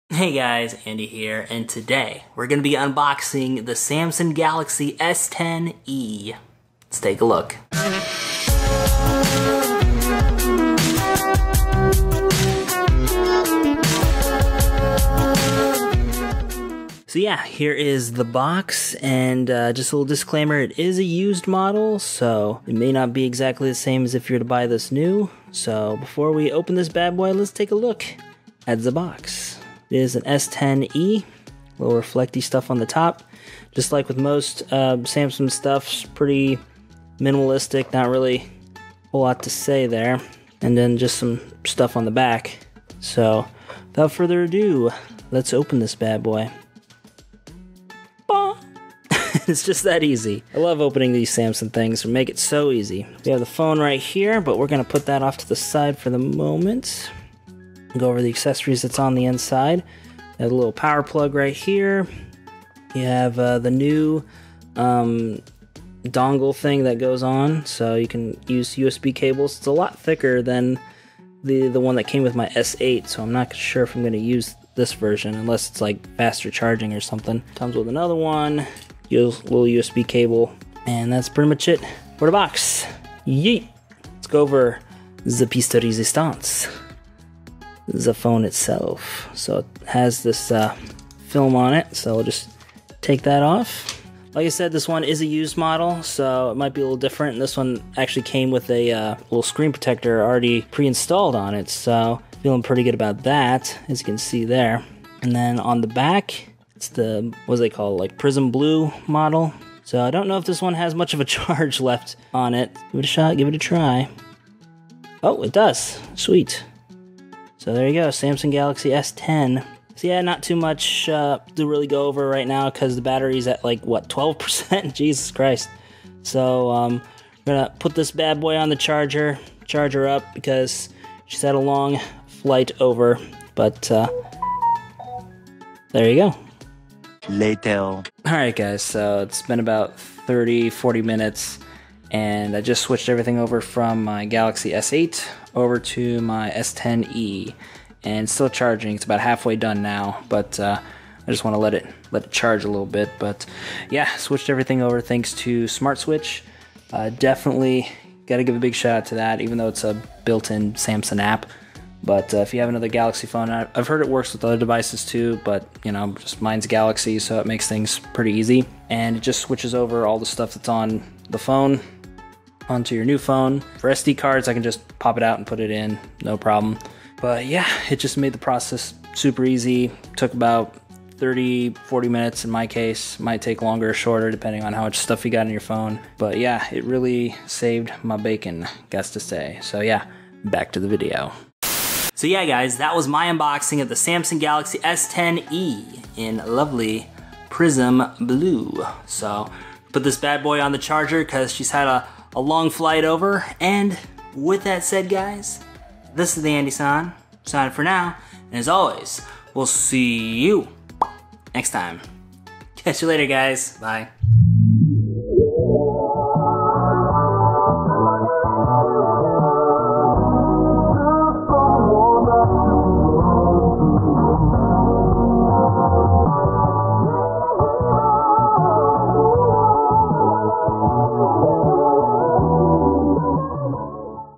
Samsung Galaxy S10e Unbox
I unbox my newest cell phone in exactly 2 years, the Samsung Galaxy S10e!!

Ramen Adventures

Ericsurf6

Drift Hunter

**Tech Notes**

*Camera Used*

Panasonic G85

*Mic Used*

Rode VideoMicro

*Programs Used*

The Levelator 2.1.1 (revision 4261)

*Music Credits*

BGM Artist: Dan Henig